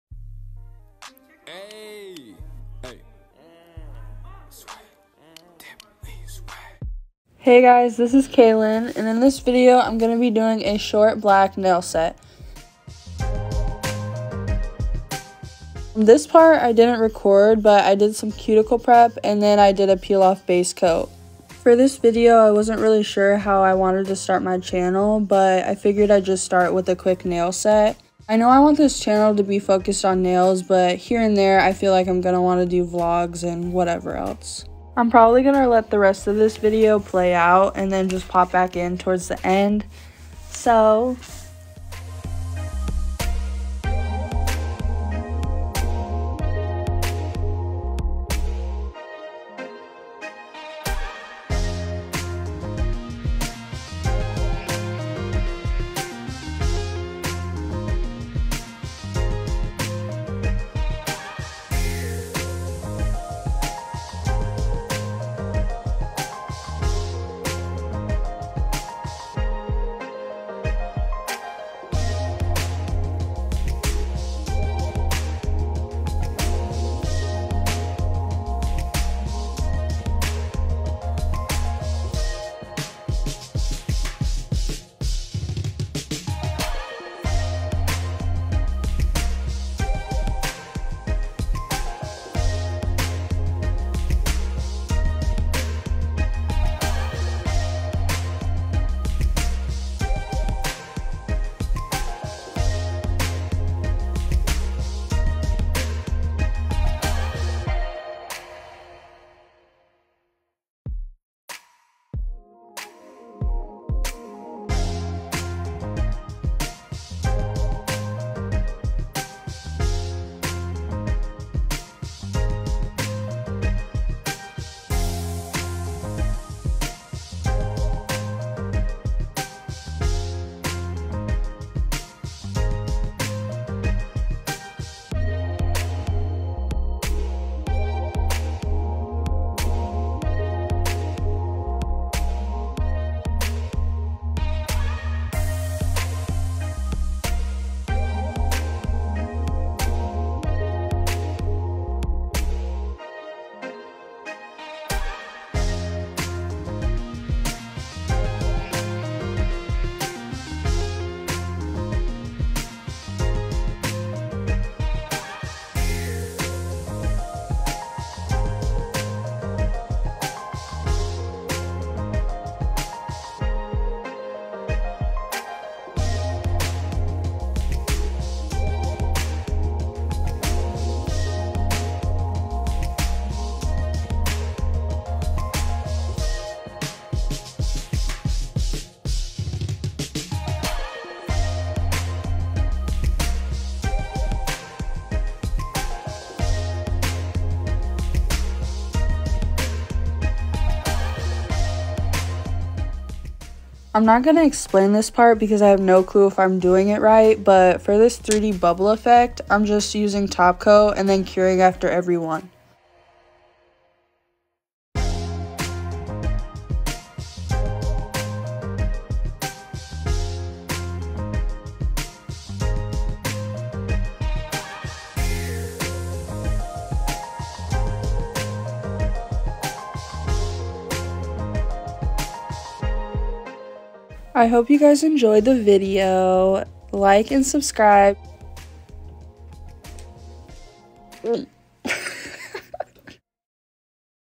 Watch me do this matte acrylic set!
In this video I’m doing a short matte acrylic set. I hope you guys enjoy!